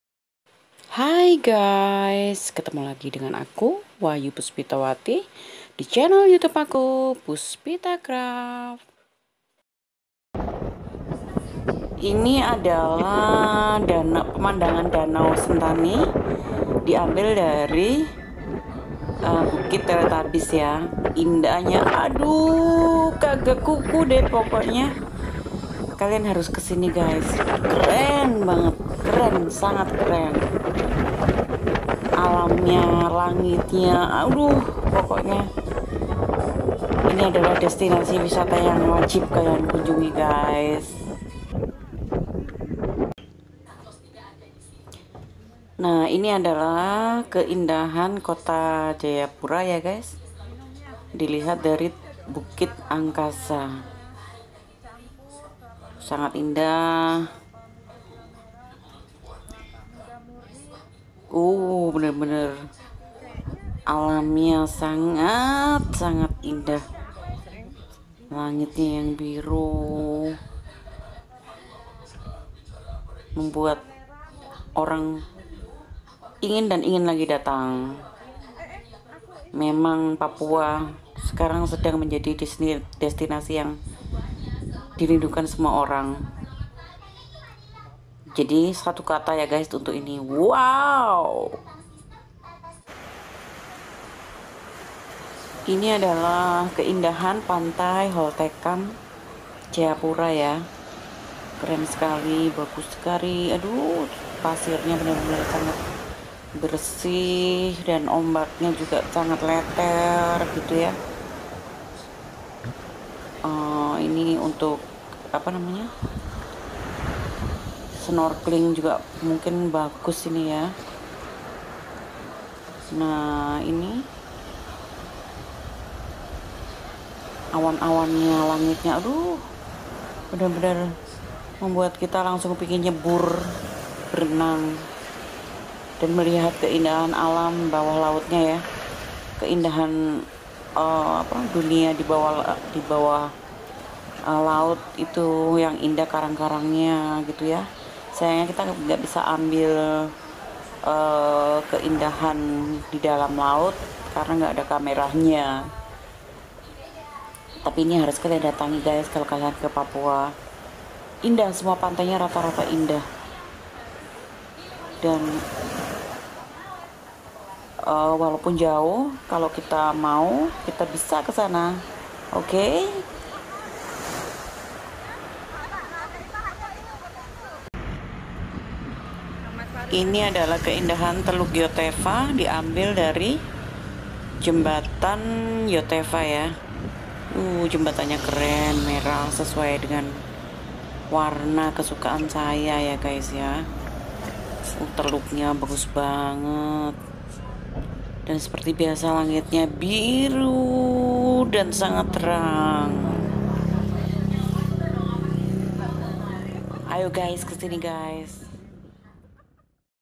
keindahan alam di Papua ...
keindahan alam di papua .. judul ini bukan klise ya, tapi memang papua benar benar sangat indah, walaupun jauh tapi terbayar dengan keindahannya, jadi judul keindahan alam di papua itu sangatlah tepat ..